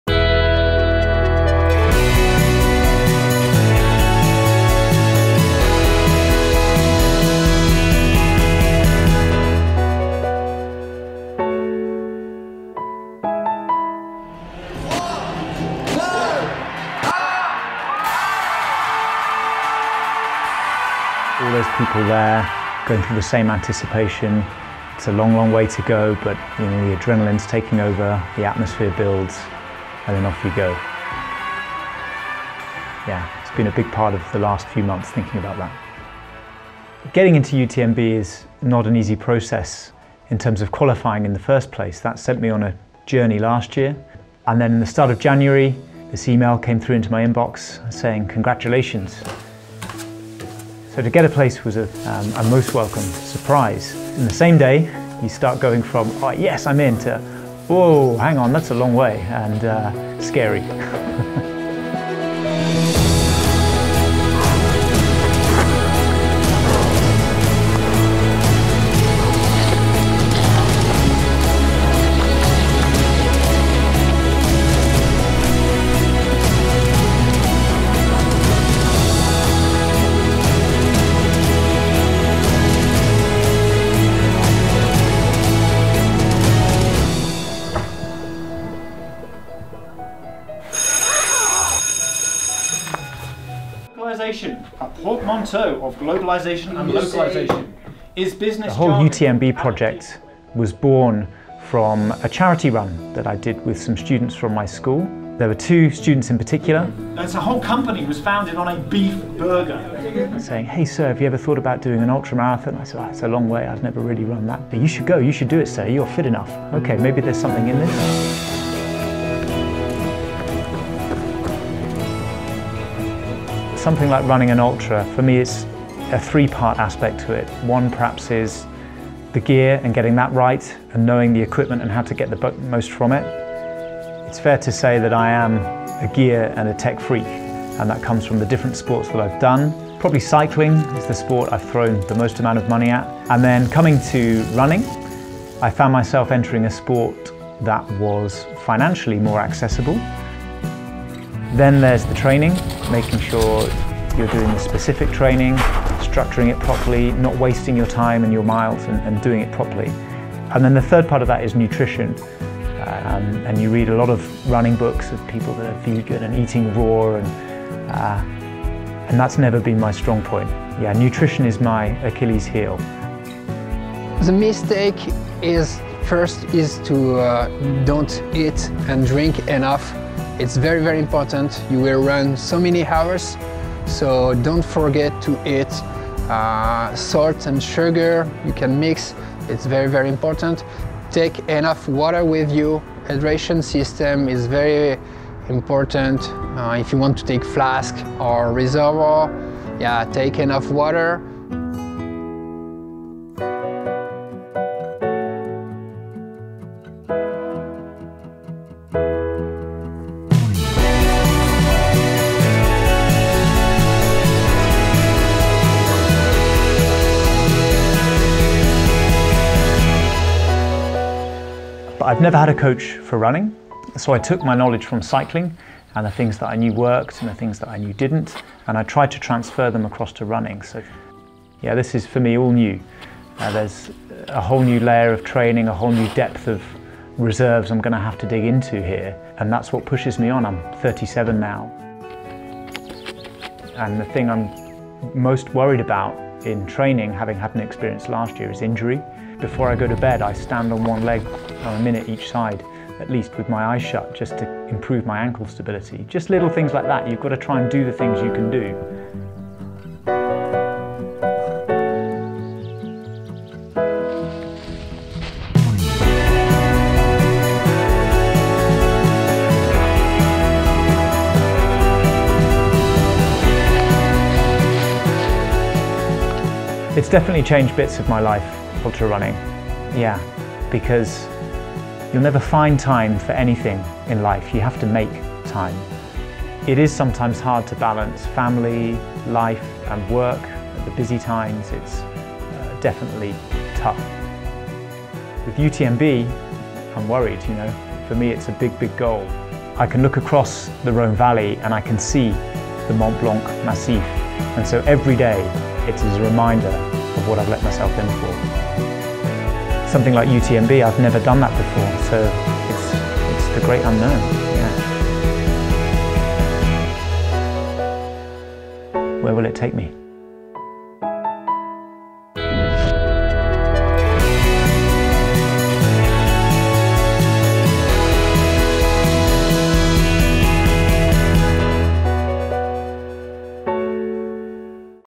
Columbia presents UTMB® Stories - My First UTMB
My First UTMB®

In his classroom, with his blue tie, Steve does not look like an ultra-trailer in full preparation for his first UTMB®. At the age of 37, he needed a new sporting challenge! While awaiting the next edition of UTMB on the 1st of September 2017 in Chamonix, he’s stacking the hours of training in his busy professional and personal planning. A challenge in the challenge!

Eyes on the starting line of the UTMB, Steve managed to reorganize his life around this race. He has three goals: gold, silver and bronze. « Bronze is to finish! Silver: be in the first half of the ranking. My personal gold prize? Finish in 27 hours ... 27 hours because after that it’ll be too late for my family to welcome for me on the finish line and I would love to live this unique moment with them! ".

A fourth goal, which does not come in any kind of metal, reveals a more personal dimension of his quest: "there is an addictive element in the ultra-trail, that is the big question I ask myself and to which I still have no answer: how far can I push my body? How far can I go? The UTMB® is a test: my two legs, a fantastic tool, where will they take me? ".